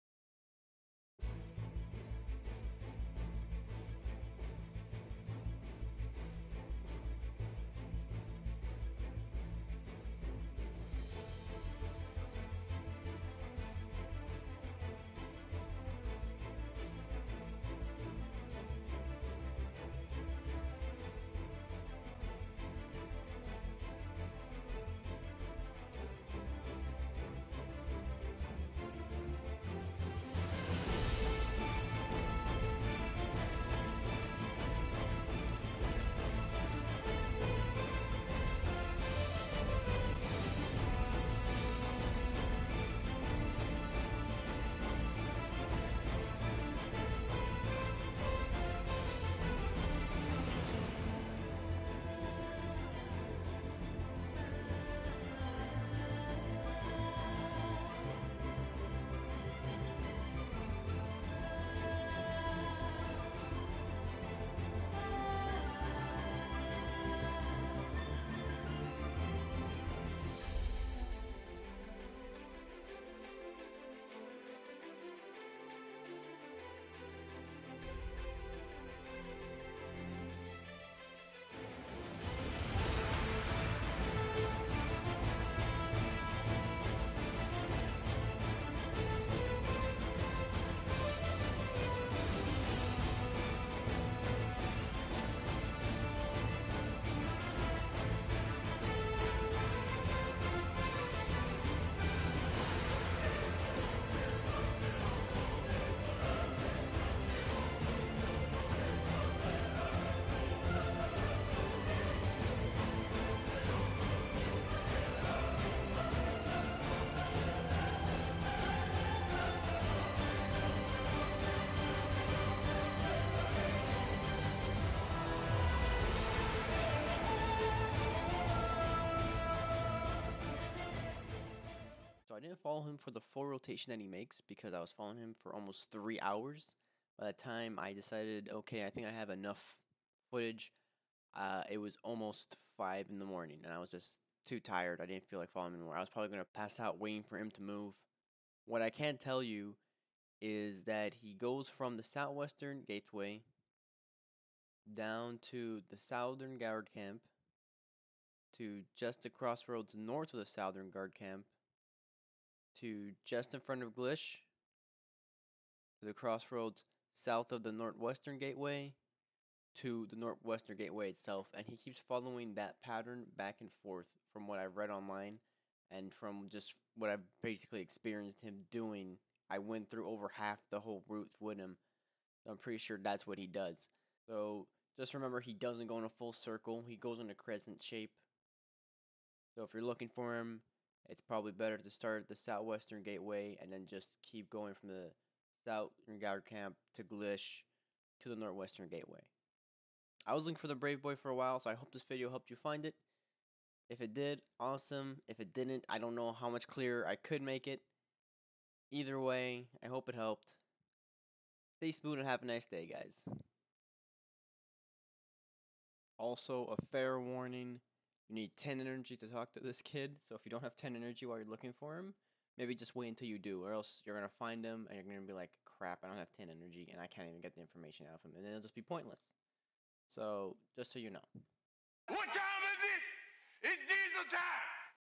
Tracking Brave Boy! People of Serendia
SKIP TO ABOUT 2:30 IF YOU JUST WANT THE AUDIO RUN DOWN!
This guy seems to be a common piece people are missing. Hopefully this video helps him become easier to find!